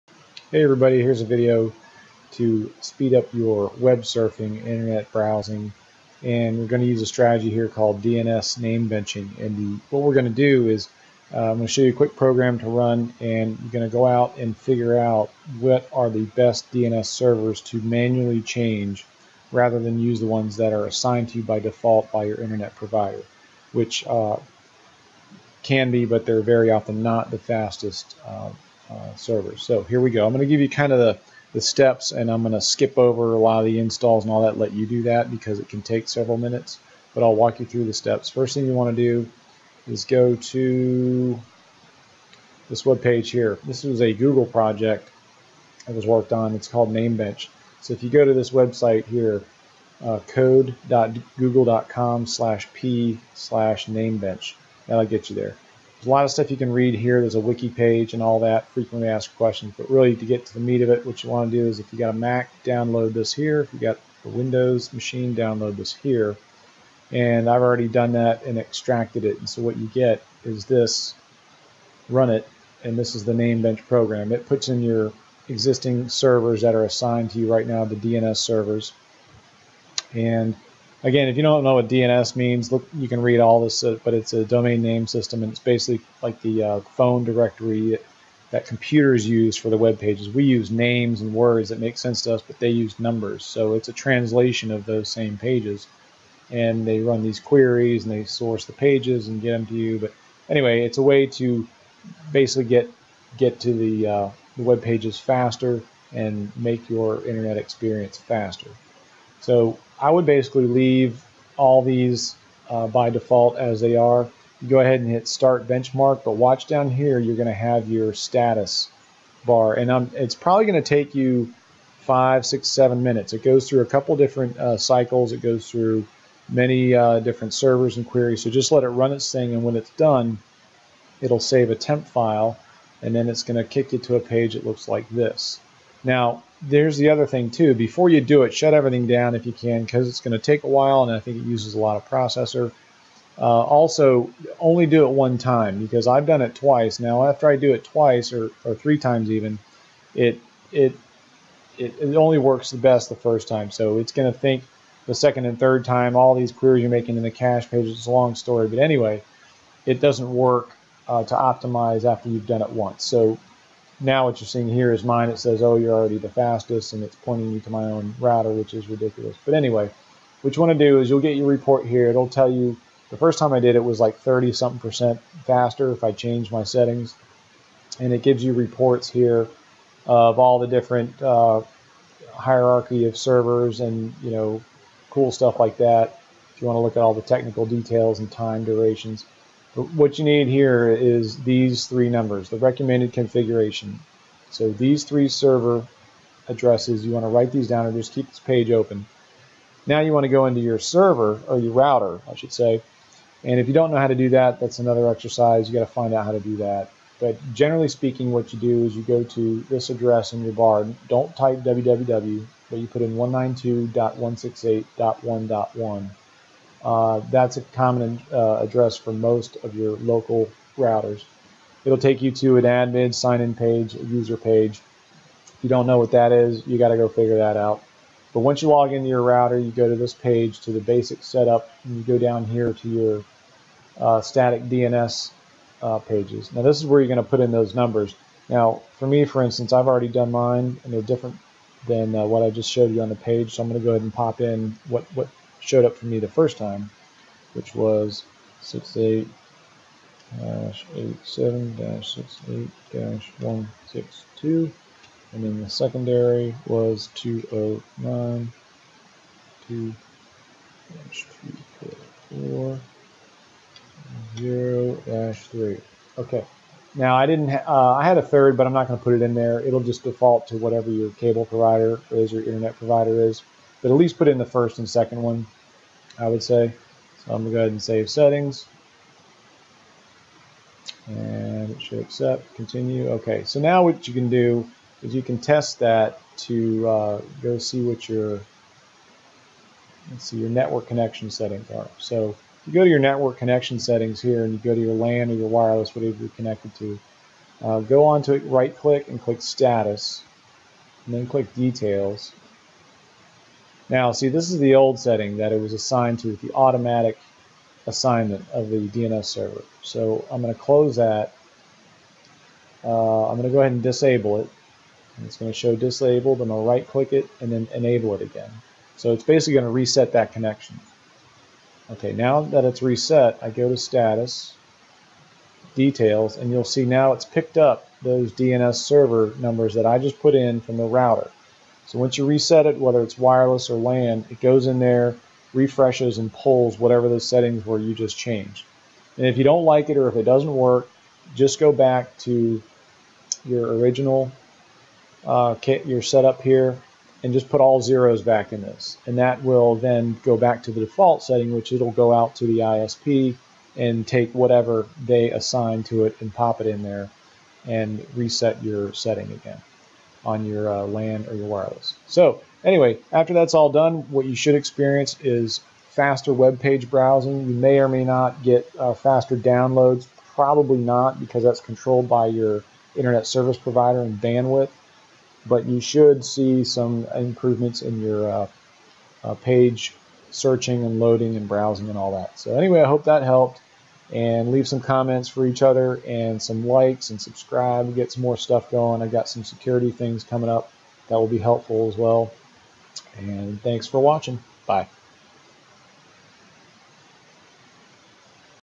Change THIS to Make Your Internet Surfing FASTEST
If you want to maximize your speed in browsing the web and surfing the faster  internet, do this optimization test and setting change on your DNS setting.  I did it for mine and it said the DNS server was about 30% faster for me.  You don't have to get stuck with the DNS servers that are assigned by your big ISPs!

You can do this change at the router level OR at an individual device network connection setting (like wireless or LAN connection for a pc).

Help each other out man!